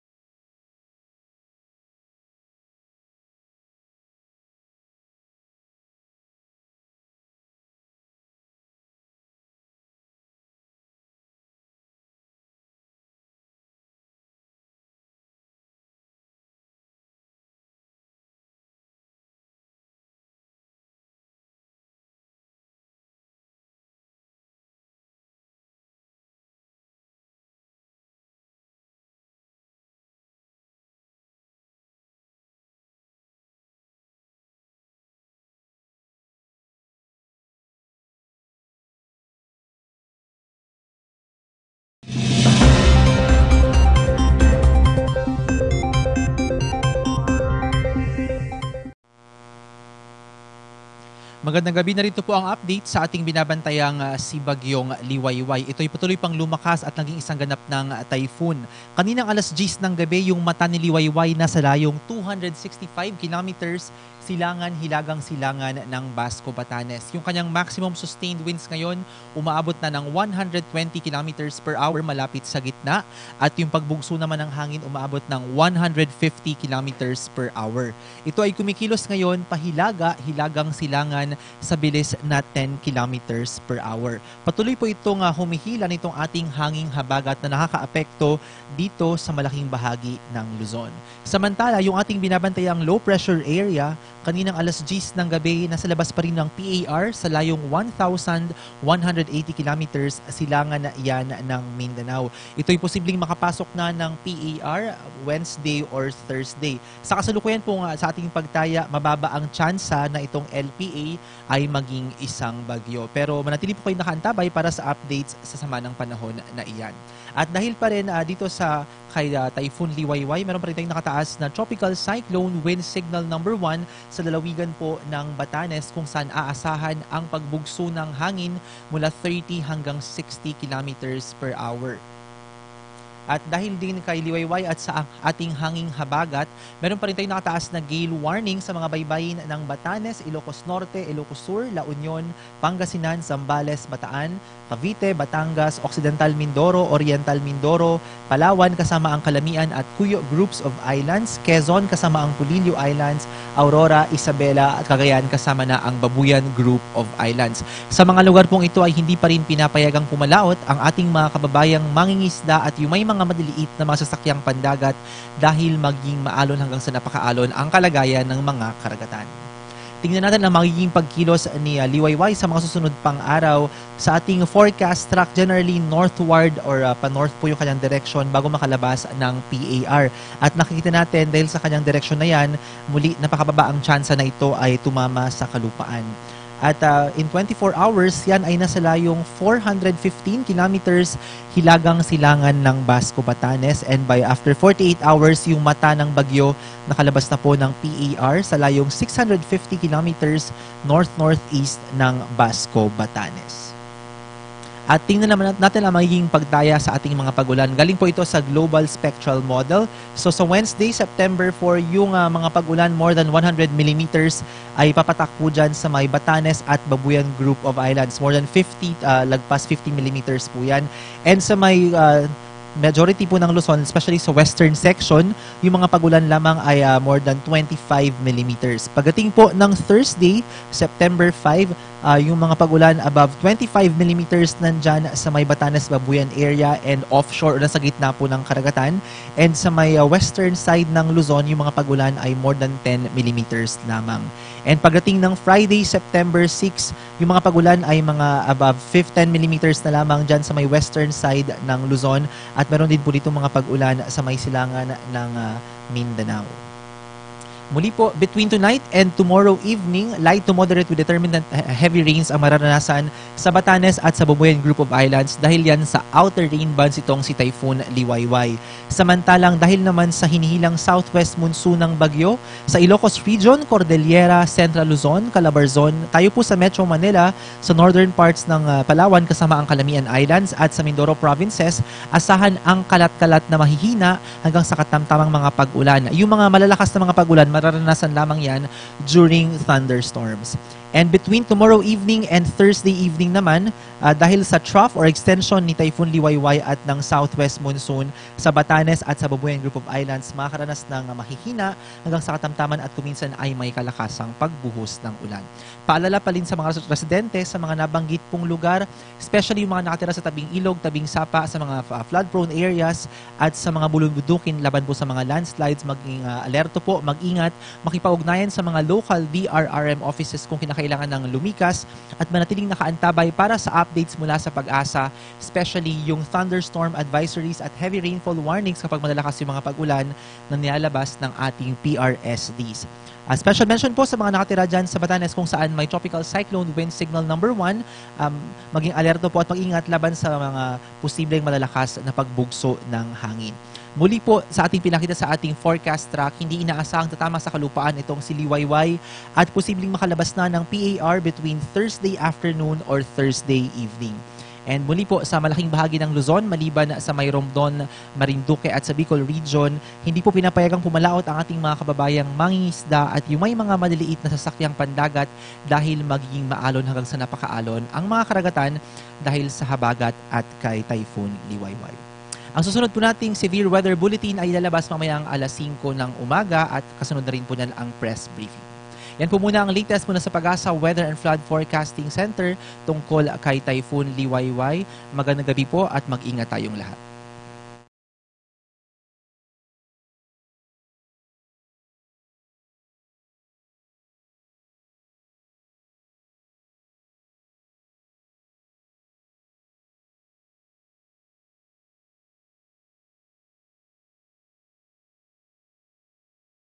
Press Briefing: Typhoon "LIWAYWAYPH" Tuesday, 11 PM September 3, 2019
DOST-PAGASA Weather Specialist: Ariel Rojas